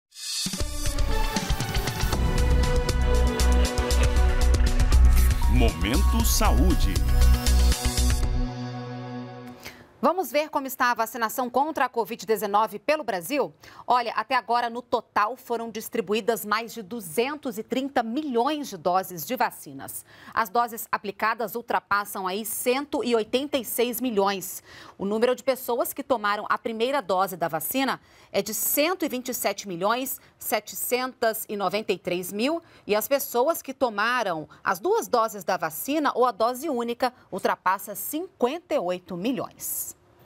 Vacinômetro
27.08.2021 – Até agora, o Ministério da Saúde já distribuiu mais de 230 milhões de doses de vacinas contra a covid-19.